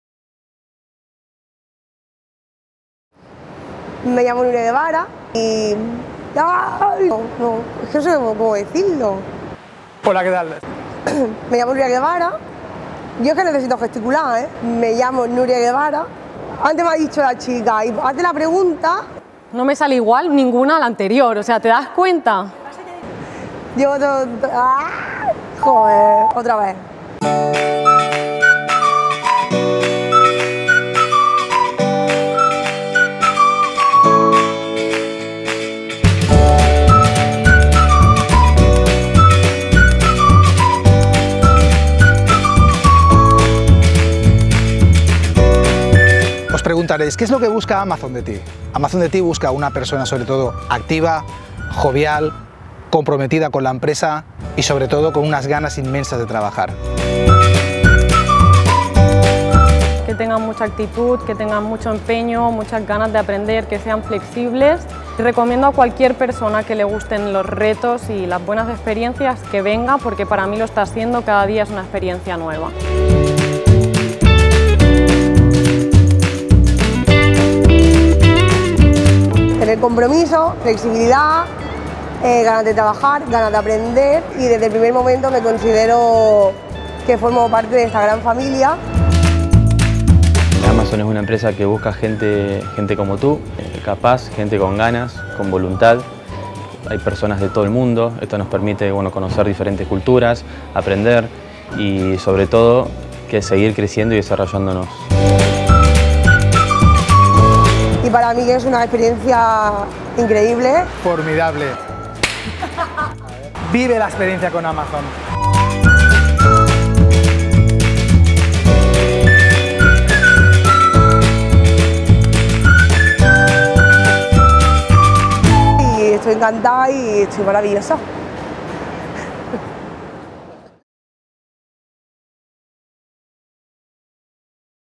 ¿Sabes qué hay entre el clic y el paquete de Amazon que te llega a casa? Personas como tú
Conoce a Nuria, Xavi, Ana y Nico… ¡y únete a nuestro equipo!